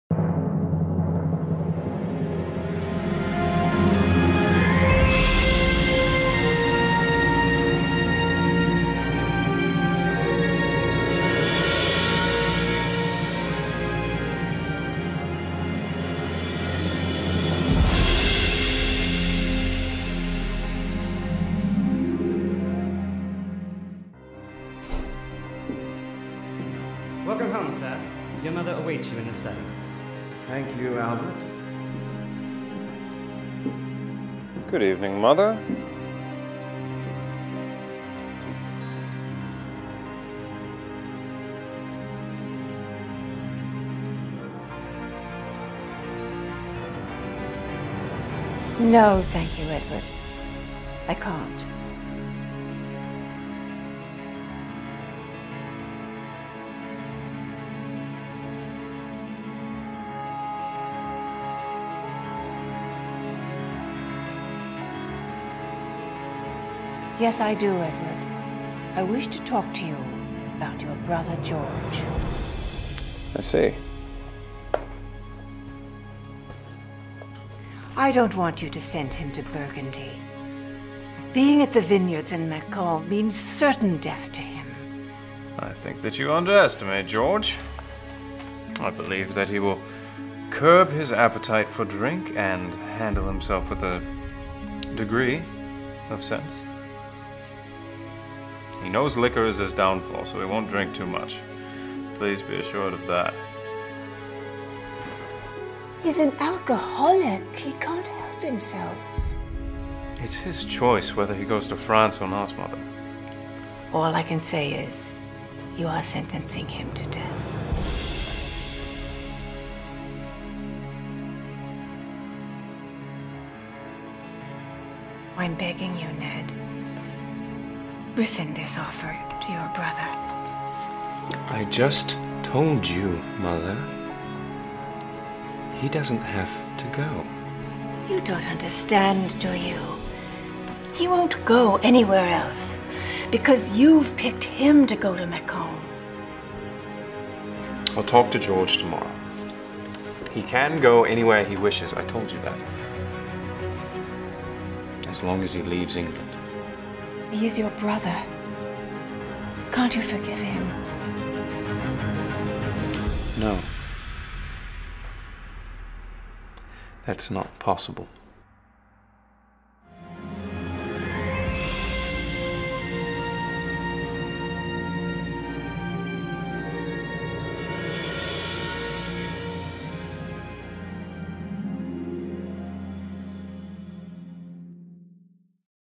The Heir - Scene 3
Scene from the latest book by Barbara Taylor Bradford's, "The Heir".